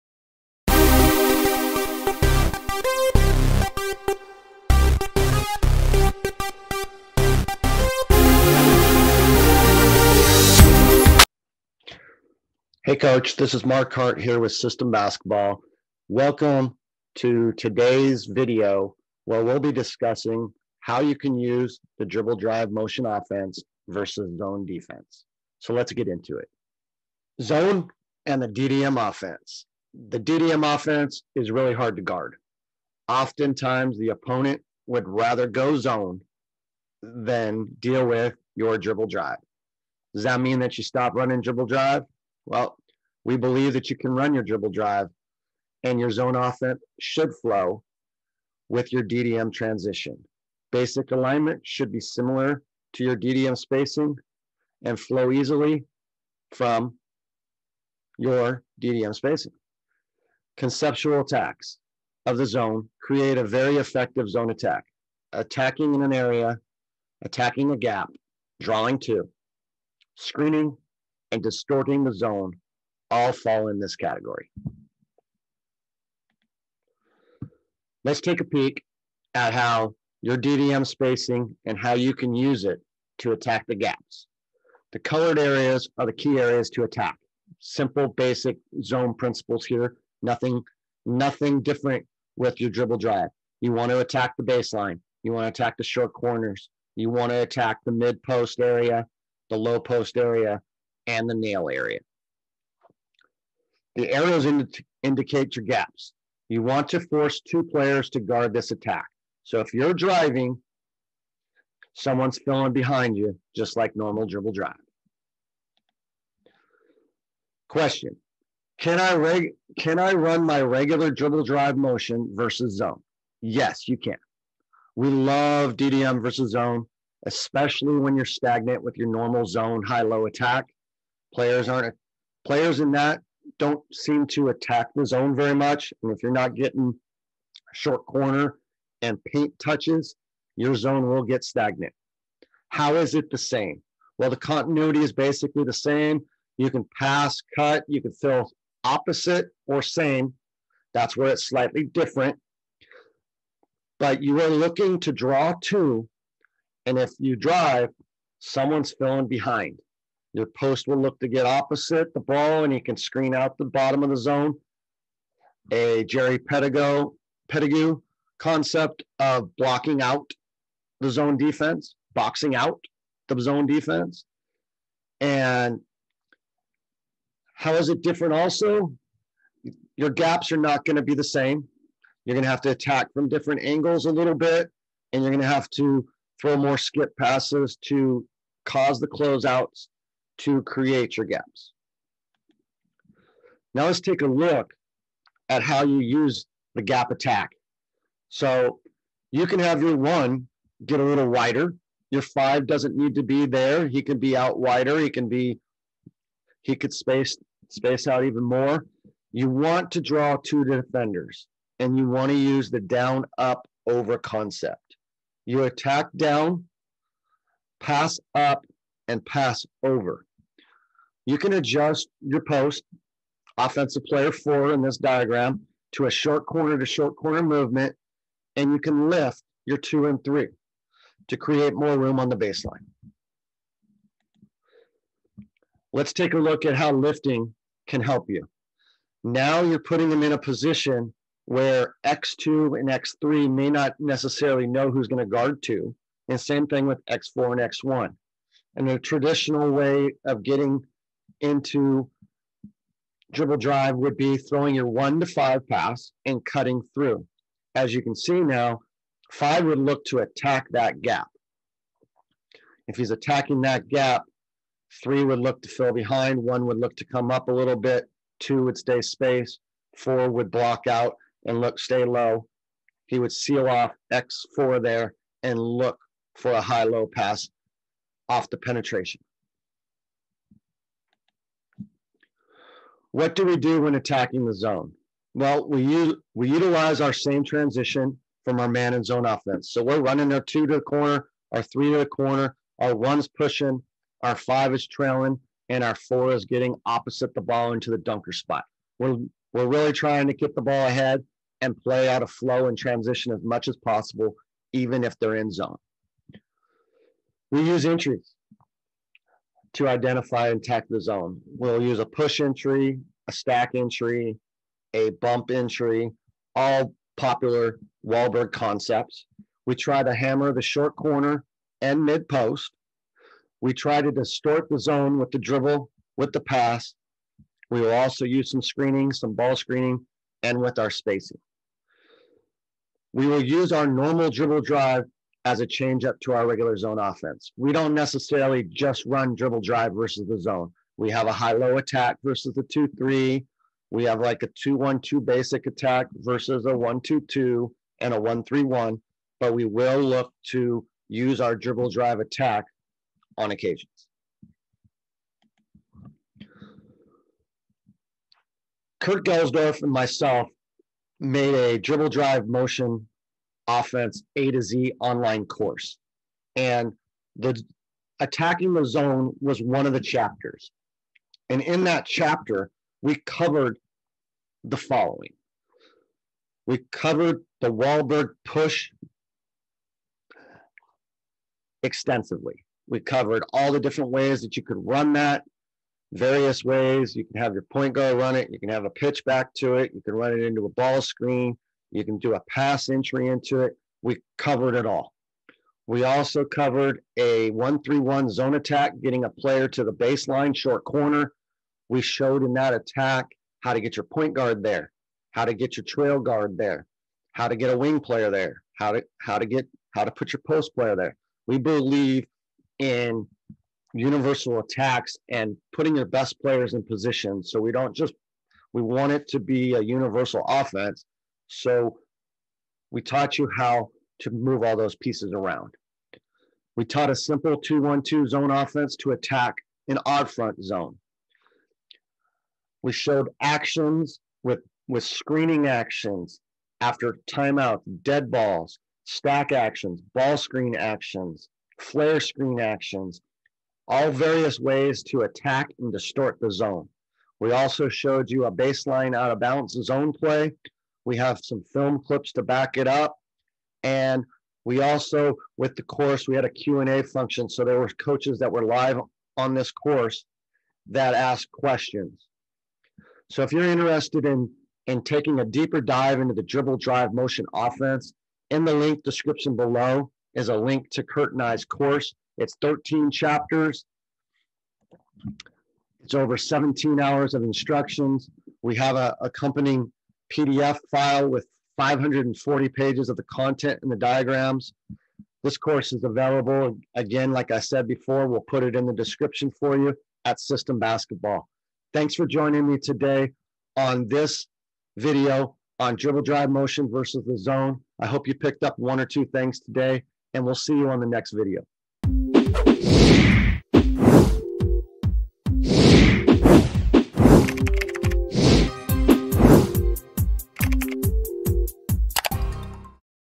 Dribble Drive Motion Offense vs. Zone Defense
Dribble Drive Motion Offense vs. Zone Defense. Coach Marc Hart discusses how he uses the Dribble Drive Motion (DDM) to attack zone defenses.

Coach Hart has been running some form of the Dribble Drive Motion offense since 2008.

💻 Dribble Drive Motion Offense A to Z Course with Marc Hart and Kurt Guelsdorf

🎥 Table of Contents:
0:00 Intro
0:29 Zone and the DDM Offense
1:17 DDM Spacing and Gaps to Attack
1:57 Can I run my regular DDM vs. the zone?
3:13 DDM vs. the Zone
4:42 What do we do when attacking the zone
6:00 What we cover in the DDM A to Z course vs. Zone
8:21 Outro